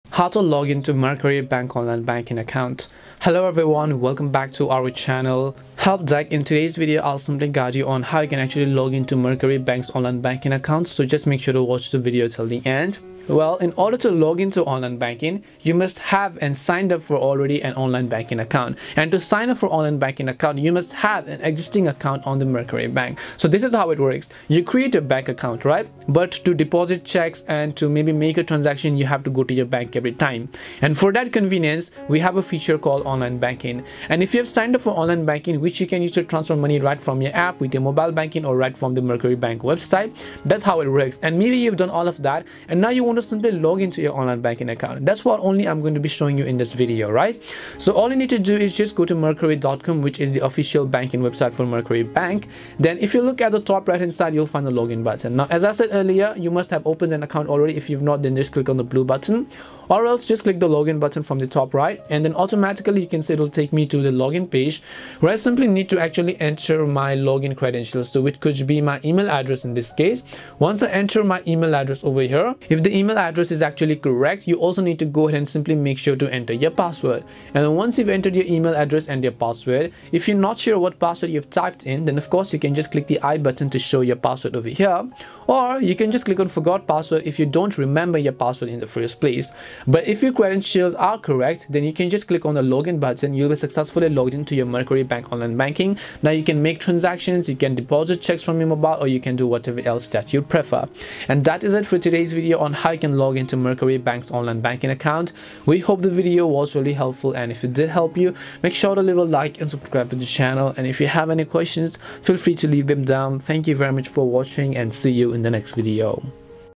Mercury Bank Login - How to Login Mercury Online Banking ?
This video guides you in quick easy steps to log in to your Mercury bank account online. So make sure to watch this video till the end.

~ Chapters:
0:00 Introduction
0:13 Official Site
0:30 Login Mercury Online Banking
1:40 Conclusion